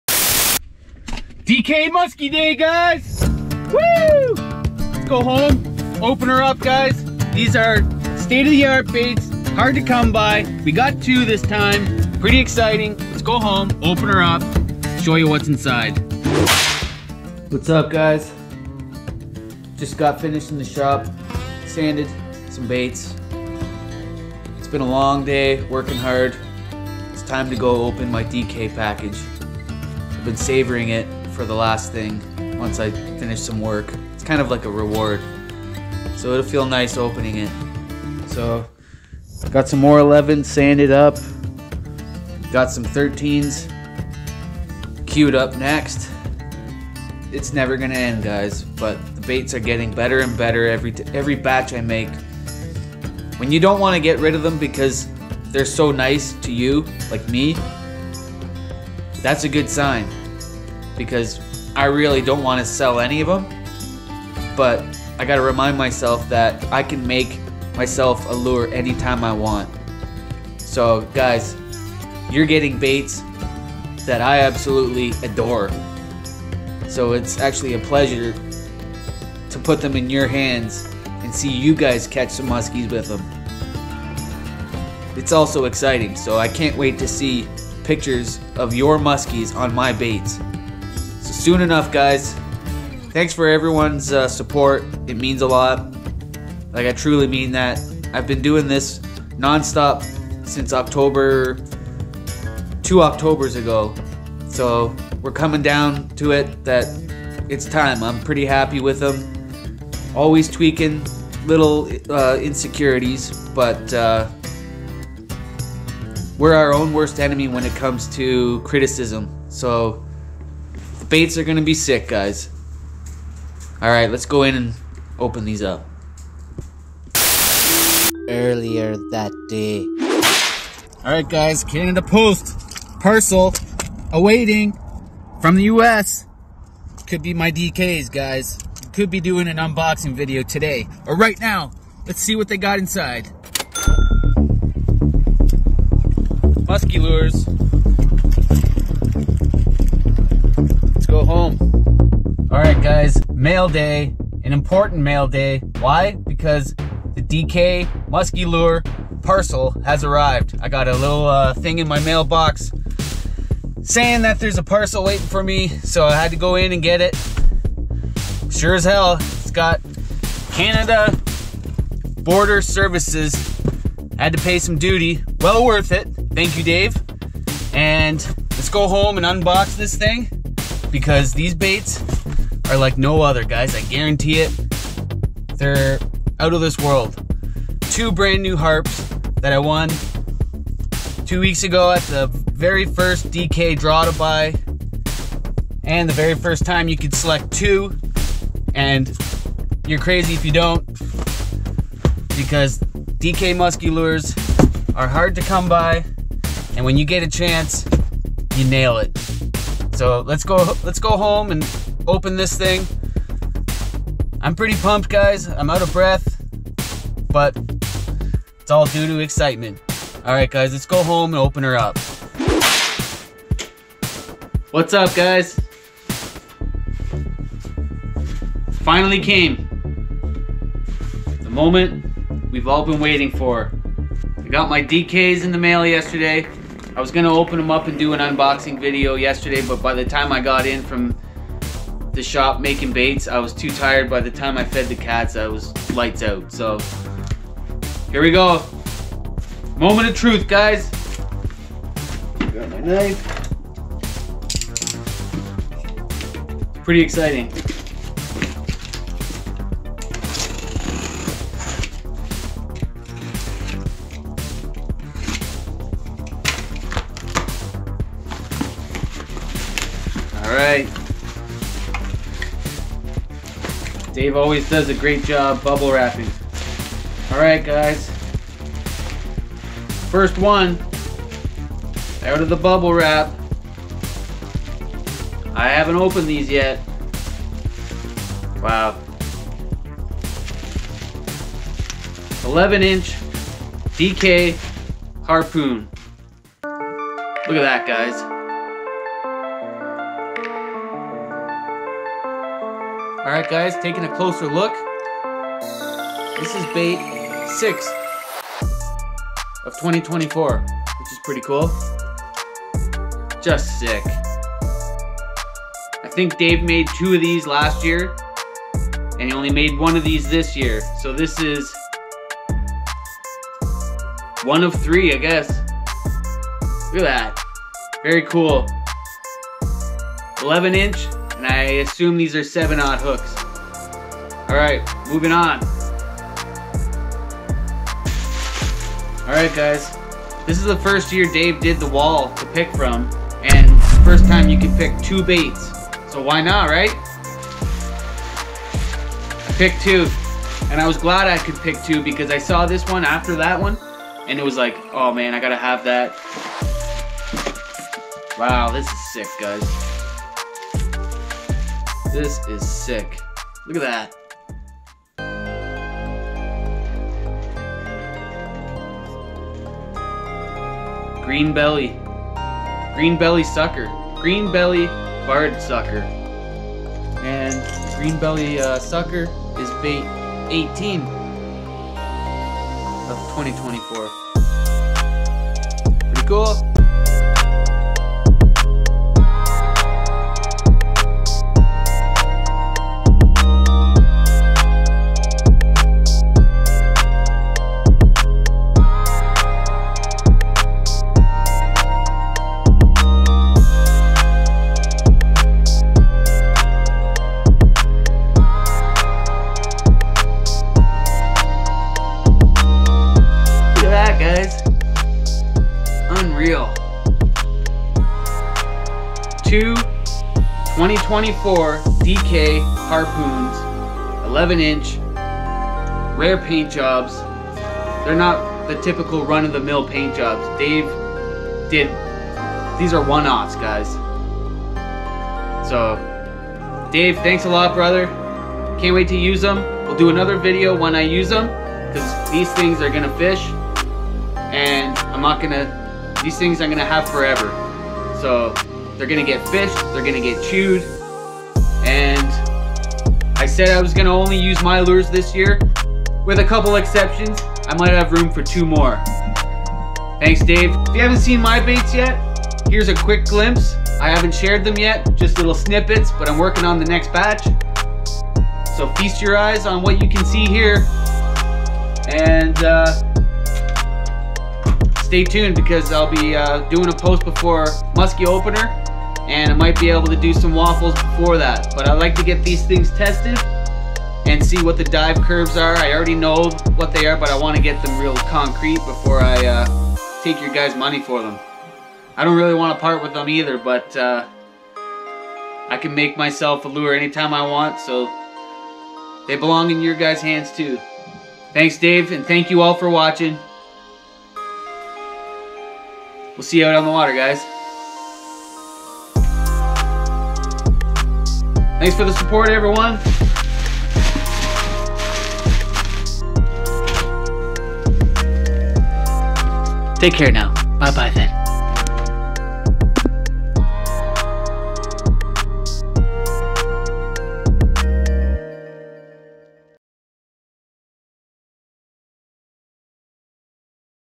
DK MUSKY LURES UNBOXING!!
Mikey gets first pick at Daves 2024 Lure Wall!!
Baits are in!
Check it out!!